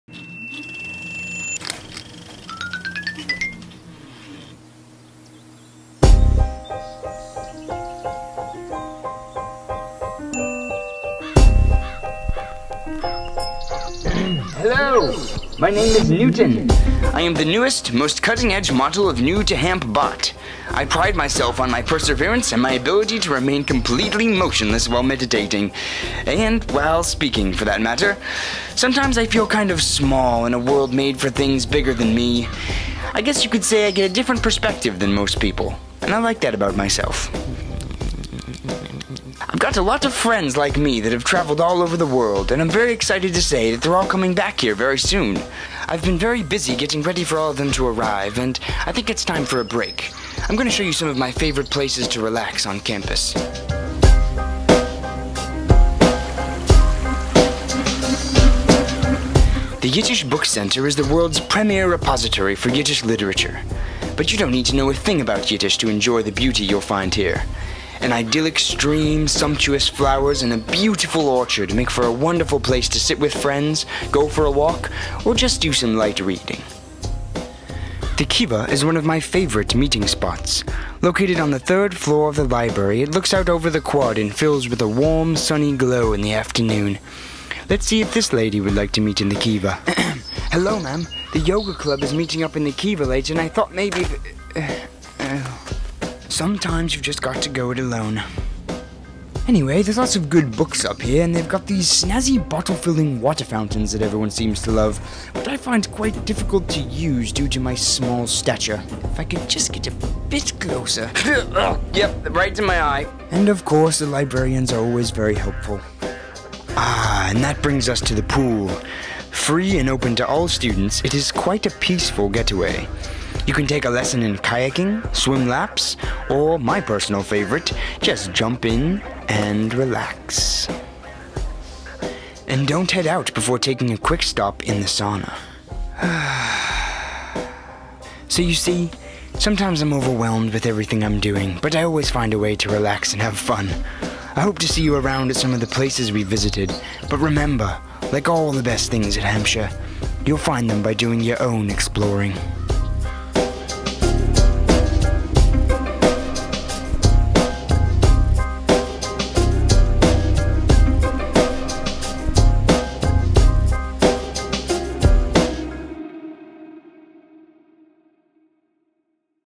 Hampshire College • Newton's Laws
This summer, the office of new student programs sent NEWTOHAMP BOT, a little robot friend, to all incoming students. Where does this fearless, cutting-edge robot go to unwind on campus? Find out in this episode of Newton's Laws.